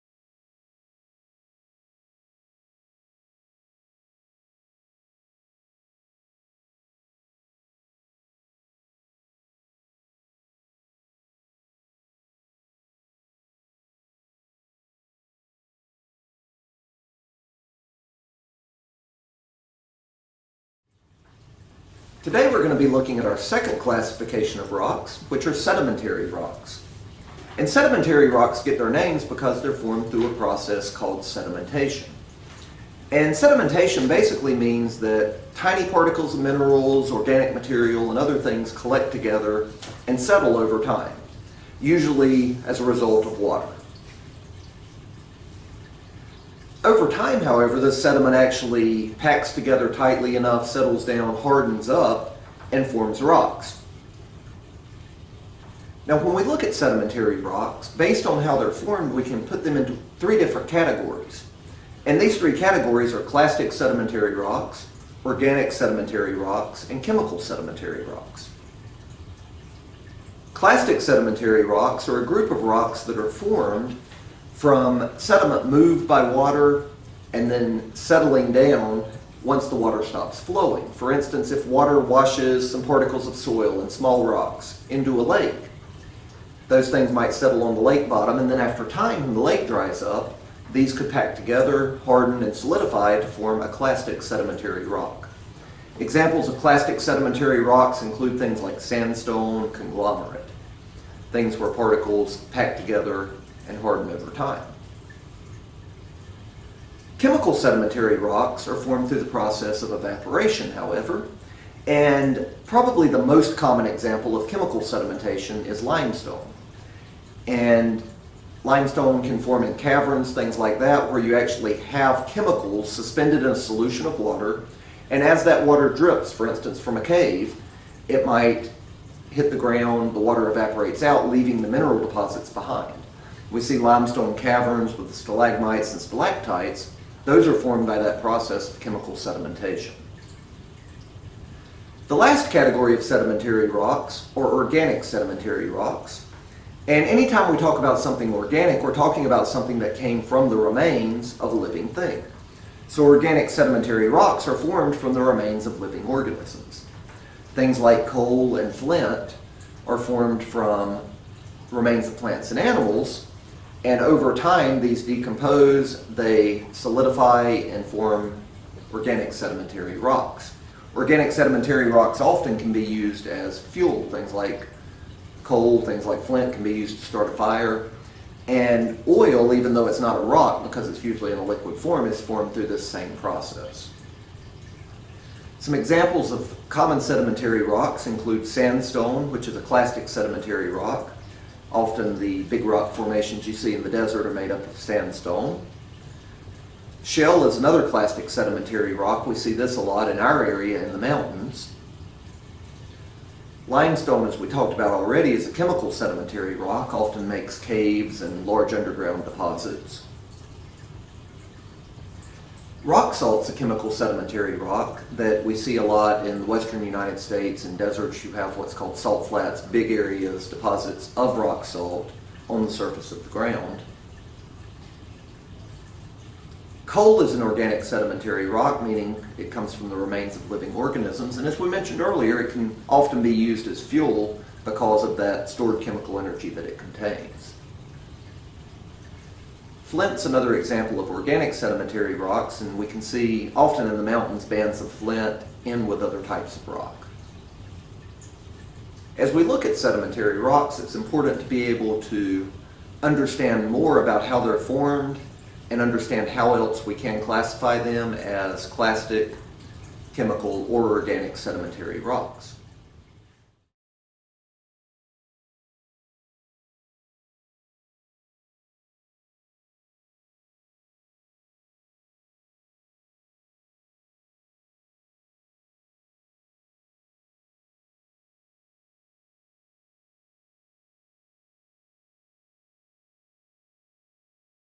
Lesson 4.3.6 - Sedimentary Rocks
In lesson 6, we will explore sedimentary rocks, our second category of rocks. Sedimentary rocks are formed over long periods of time through the processes of erosion and deposition. These rocks will often be softer than igneous rocks with distinct layers from the sediment that formed them. Because of how they form, sedimentary rocks can also contain the fossilized remains of plants and animals that become buried in the sediment before it hardens. The three types of sedimentary rocks: clastic, chemical, and organic, form differently; but all have similar characteristics. In this lesson we will examine how sedimentary rocks form and look at examples of several types of sedimentary rock. Students will also have to opportunity to model the processes by which sedimentary rocks can be formed.